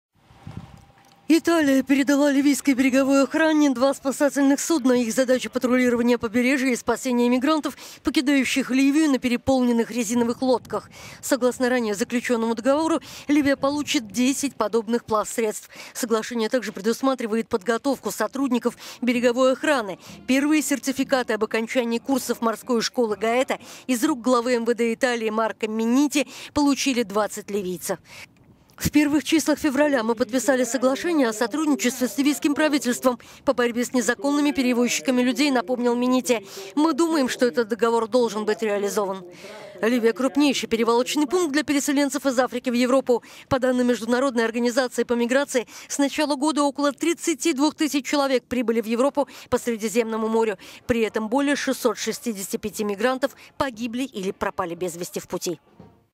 Италия и Ливия объединяются в борьбе с нелегальной перевозкой мигрантов
Италия передала ливийской береговой охране два спасательных судна. Их задача - патрулирование побережья и спасение мигрантов, покидающих Ливию на переполненных резиновых лодках. Согласно ранее заключенному договору, Ливия получит десять подобных плавсредств. Соглашение также предусматривает подготовку сотрудников береговой охраны. Первые сертификаты об окончании курсов морской школы Гаэта из рук главы МВД Италии Марко Миннити получили 20 ливийцев. На церемонии присутствовал посол Ливии в Италии …